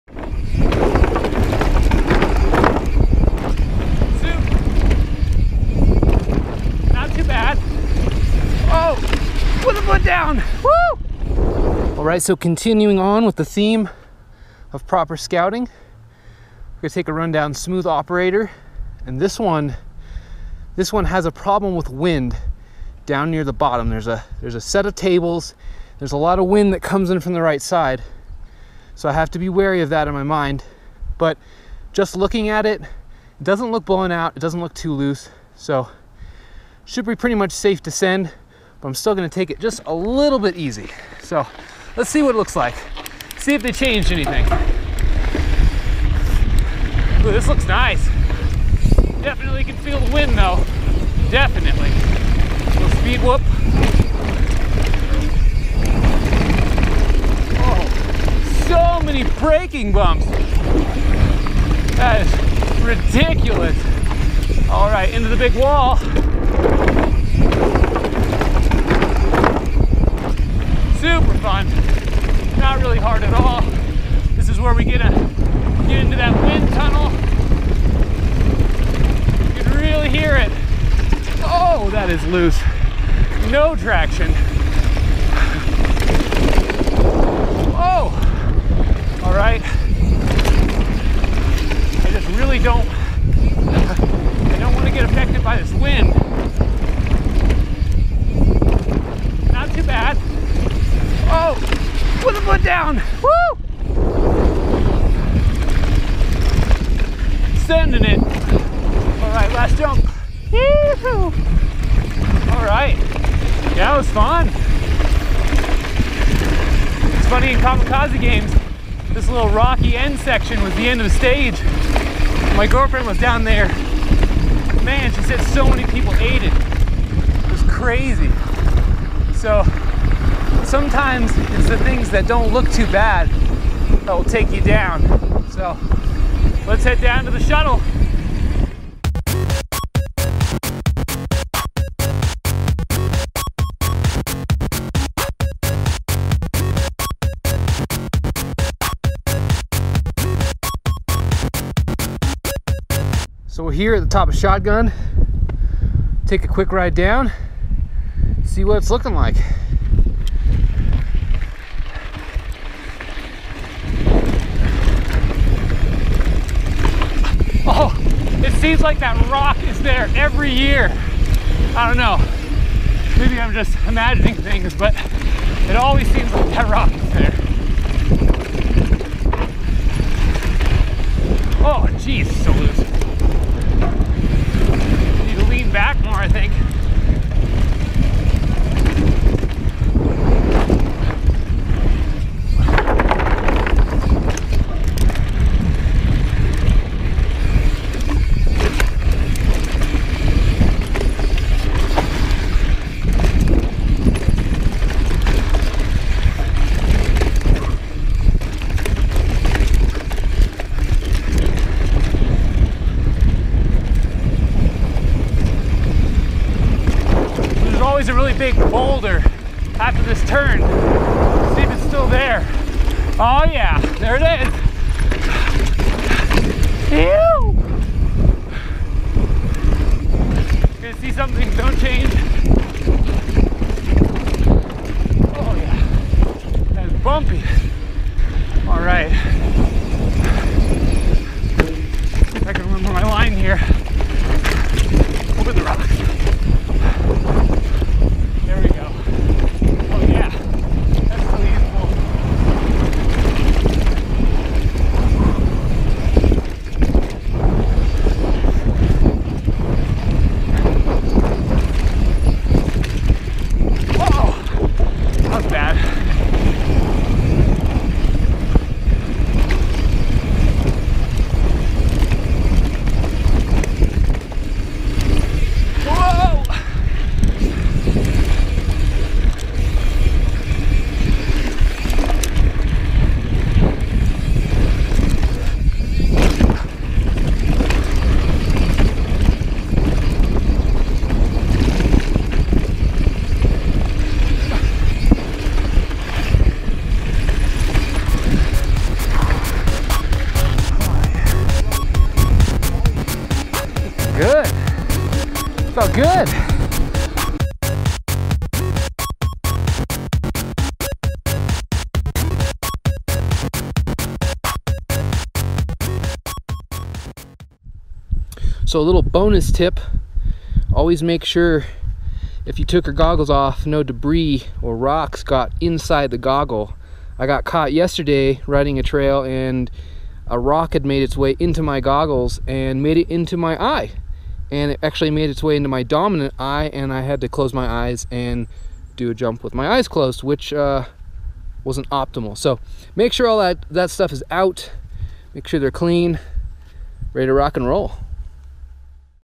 Shotgun | Smooth Operator | Mammoth Bike Park | 4K
First runs continue for the 2018 season! The top of the mountain isn't open yet, so this is the best chance to do some laps on Shotgun and Smooth Operator.

I normally session everything and get the camera out once I feel I have all of my lines dialed. On this trip I decided to try something slightly different and film my first runs on each trail.

I'll be back with more videos that are top-to-bottom pinned runs, but I thought it might be cool to show how I go about dropping for the first time.

Hope you enjoy the ride!

Camera Gear
Go Pro Hero 4 Black | 2.7k (Neutral WB)
Zhiyun Rider-M Gimbal

Bike Details
2018 Rocky Mountain Instinct
RIDE-9 in the position 1 (slack)
2018 Fox 34 (MRP Ramp Control Installed) 75 psi
2018 Fox DPS 176 psi
NOX Farlow 29 Wheels  (DT350 high engagement hubs)
Maxxis DHF 2.5 (f)
Maxxis High Roller II 2.3 (r)
Cushcore installed F/R
Sram GX Eagle
Truvative Stylo Cranks (32t ring)
RevGrips Pro, Waffle Grip
Spike Spank Vibrocore Bars 784mm
Hope E4 Brakes 203mm/180mm
Chromag Scarab Flats
HT X2 Clips
Fox Transfer 150mm Dropper
Wolftooth Drop Lever